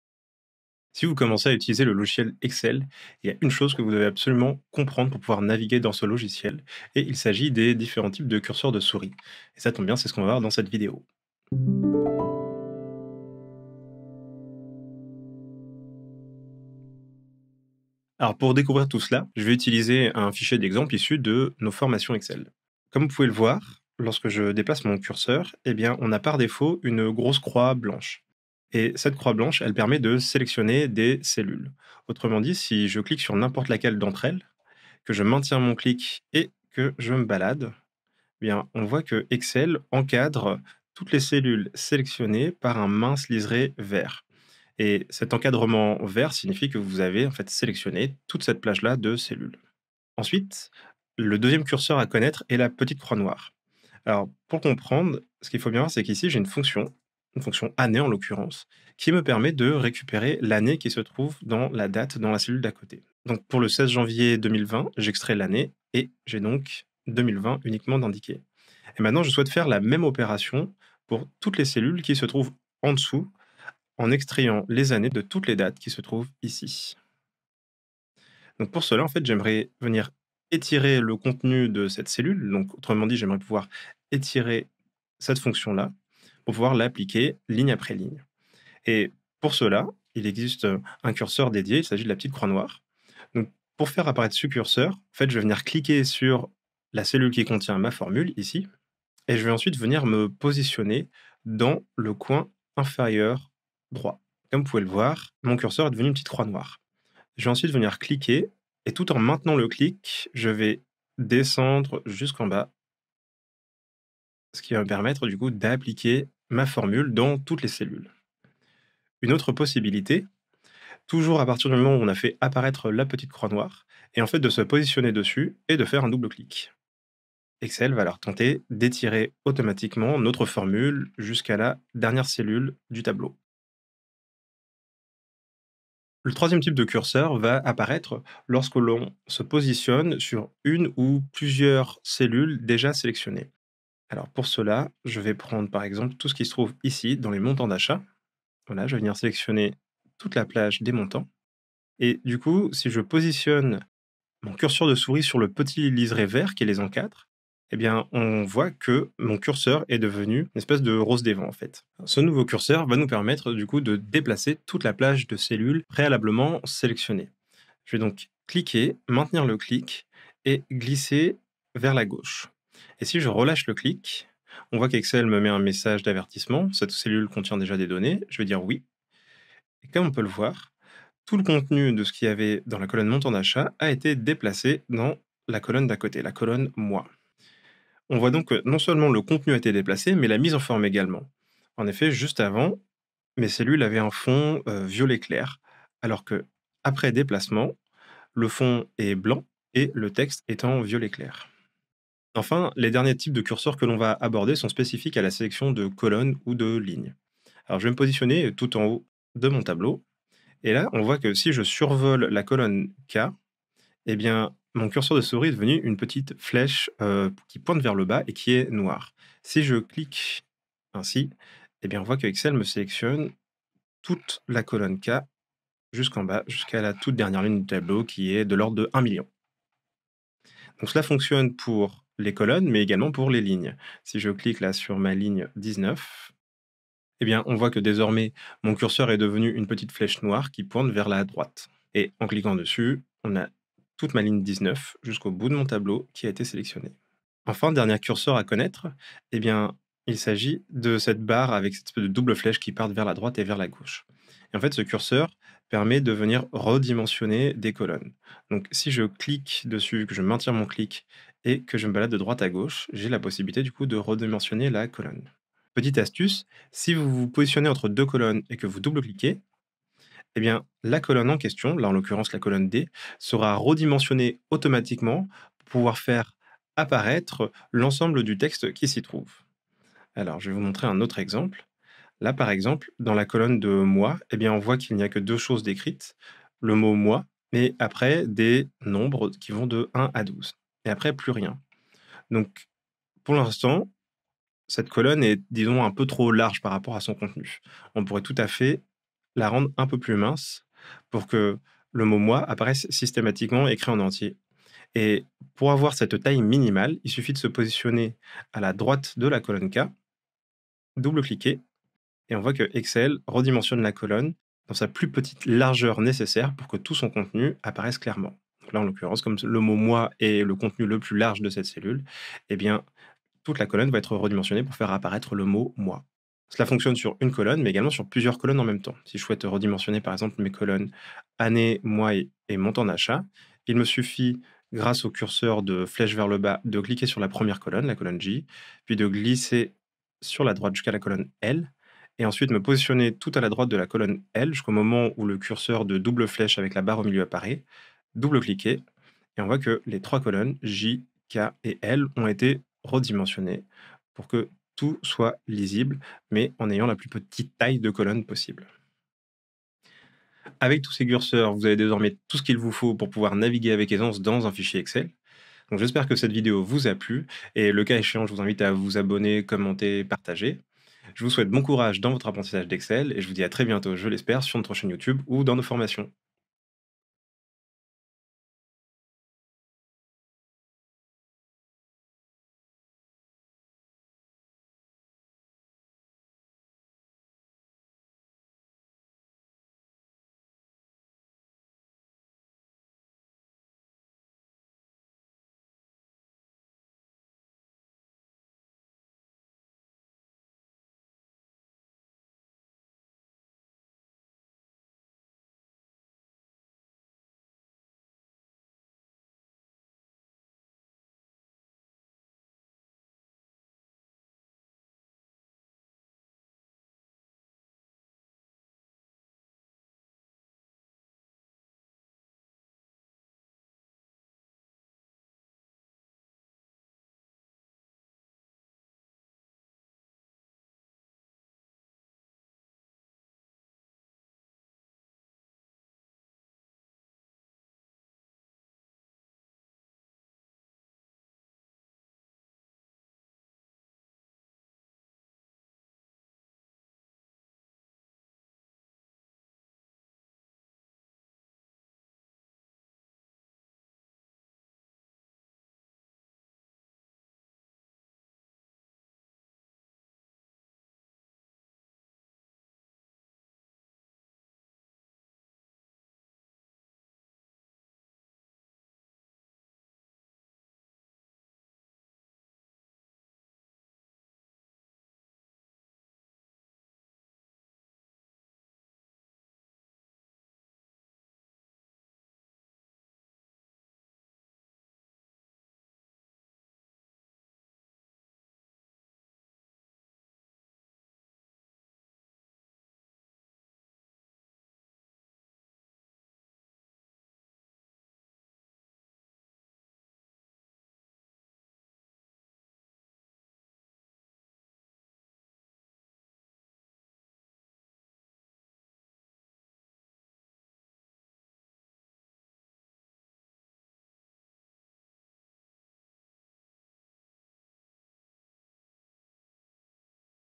LES CURSEURS DE SOURIS EXCEL
Excel est riche en fonctionnalités. Et cela se voit dès le curseur de souris : celui change de forme et de couleur selon le contexte. Nous les passons donc ici en revue afin de clarifier leurs utilisations !

0:00 Introduction

0:24 Sélectionner des cellules avec la CROIX BLANCHE
0:50 Etirer une formule avec la CROIX NOIRE
2:15 Déplacer des cellules avec la "ROSE DES VENTS"

3:23 Sélectionner des colonnes et des lignes avec la FLECHE
4:07 Redimensionner des colonnes et des lignes avec la DOUBLE FLECHE

5:59 Conclusion
6:21 Fond noir (bug d'export, ce n'est pas volontaire !)

Ce contenu pédagogique est à destination de quiconque souhaitant s'initier à Microsoft Excel. Nos exemples se font sur la version 2019 (Windows) et ce que nous y présentons est bien souvent valide dans de nombreuses versions (Windows & Mac). Dans le cas contraire, nous le précisons.